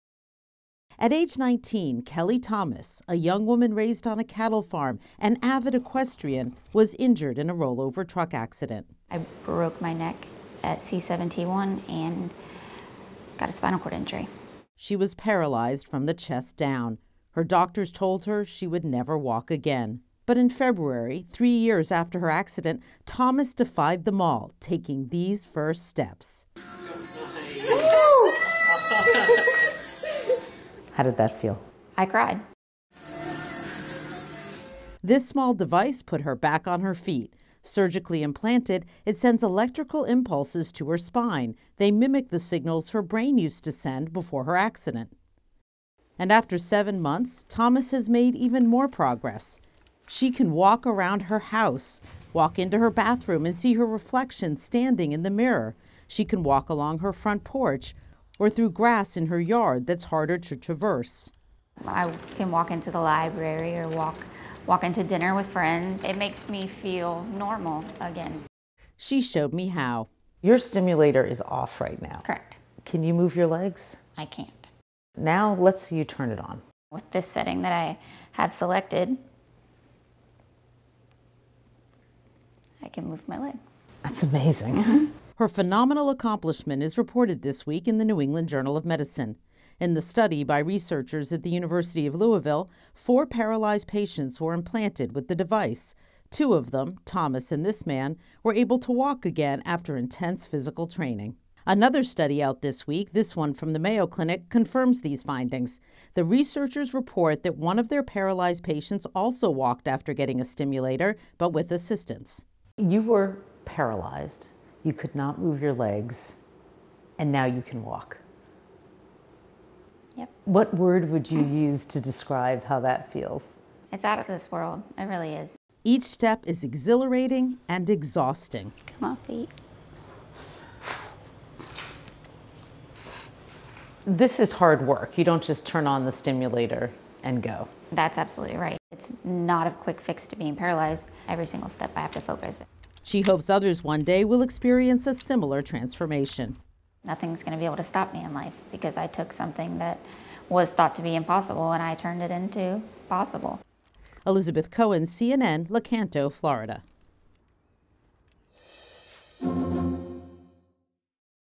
علاج رائع يساعد المشلولين للمشي مرة أخرى
بيّن فريقان من الباحثين علاجاً جديداً لإصابات الحبل الشوكي يمكنه مساعدة المصابين بالشلل على المشي مرة أخرى.